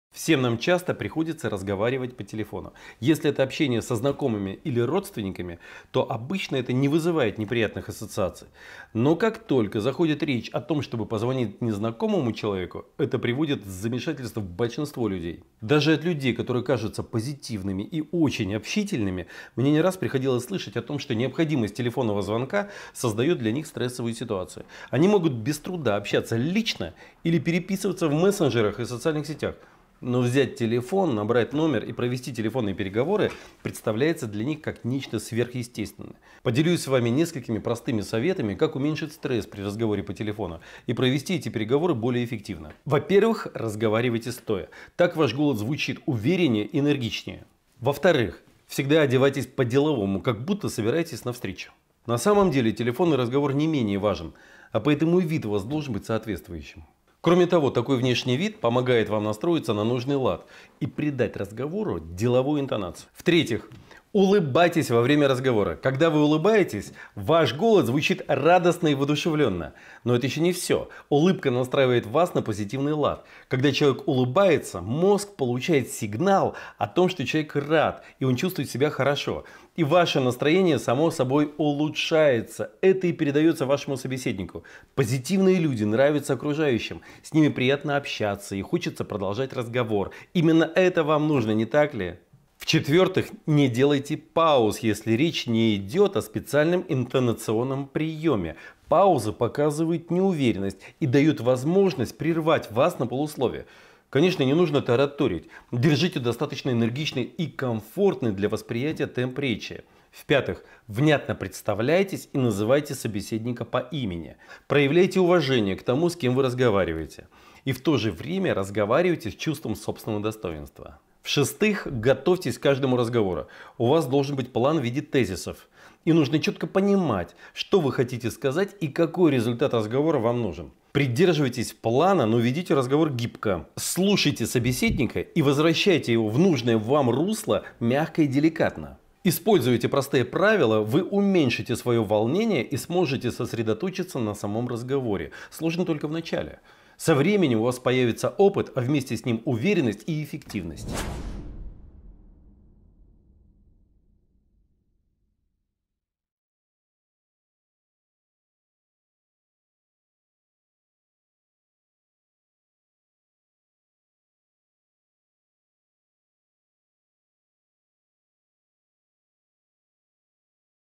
ТОП-6 лайфхаков, как разговаривать по телефону / Роман Василенко
Мы часто общаемся по телефону. Когда общение со знакомыми или родными, то обычно это не вызывает неприятных ассоциаций. Но как только заходит речь о том, чтобы позвонить незнакомому человеку, это приводит в замешательство большинство из нас. В этом видео я поделился несколькими простыми советами, как уменьшить стресс при разговоре по телефону и провести эти переговоры более эффективно.